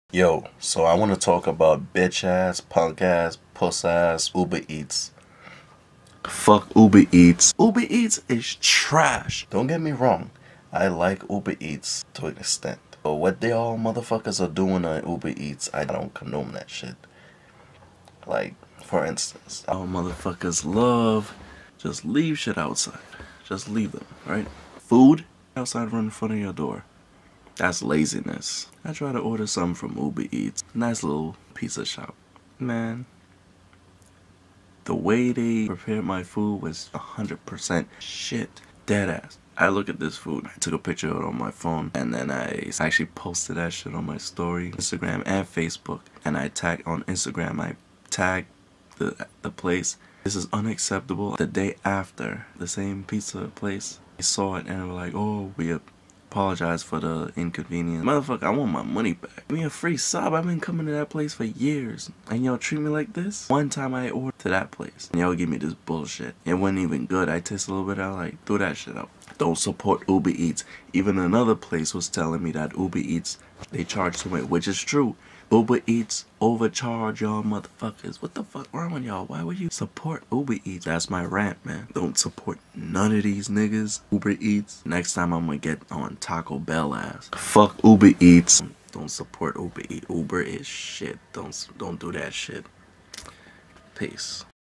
UBER EATS =TRASH!
Ya'll know what time it is. Time to roast uber eats ass, I'm tired of their shit! Also my voice sounds soft asf I didn't want to yell and wake up anyone but new rants coming soon. Hit me with the likes and tell me what you're experiences with Uber Eats and if ya'll use it.